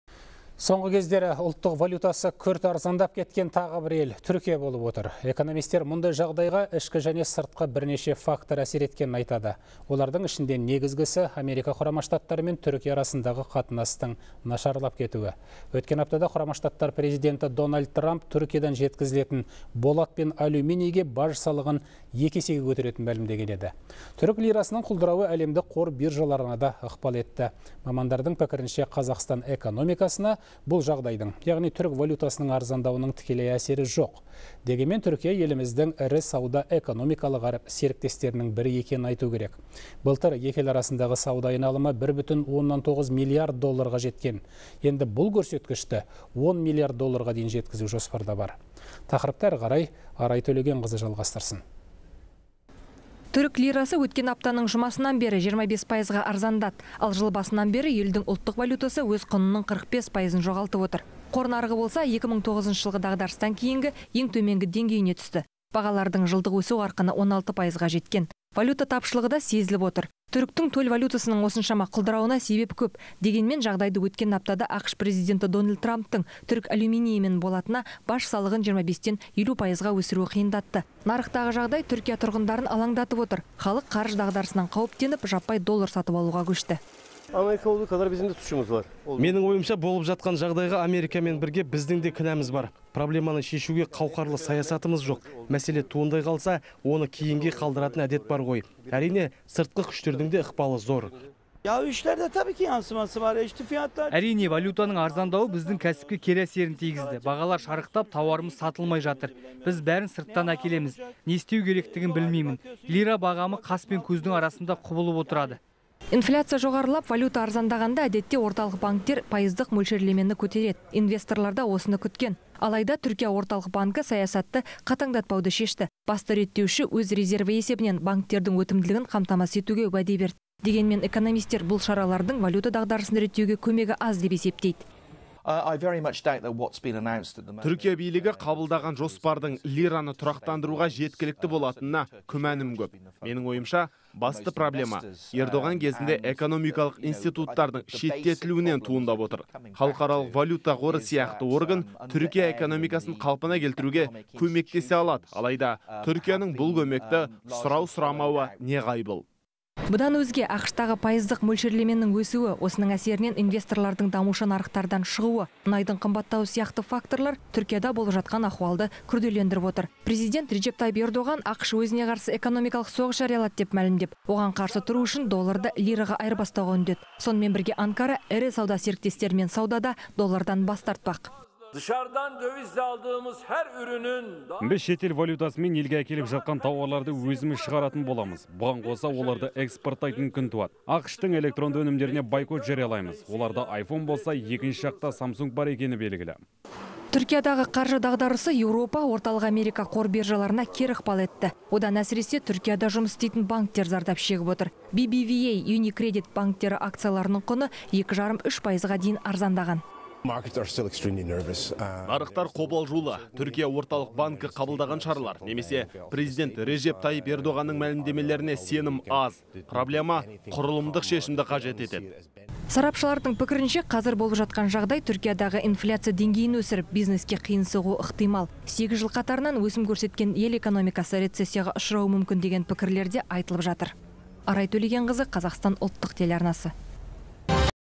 Түрік лирасы арзандады
Соңғы кездері ұлттық валютасы күрт арзандап кеткен ел Түркия болып отыр. Экономистер мұндай жағдайға ішкі және сыртқы бірнеше фактор әсер еткенін айтады. Олардың ішінде негізгісі - АҚШ пен Түркия арасындағы қатынастың нашарлап кетуі. Өткен аптада Құрама Штаттар президенті Дональд Трамп Түркиядан жеткізілетін болат пен алюминийге  баж салығын екі есе көтеретінін мәлімдеген еді.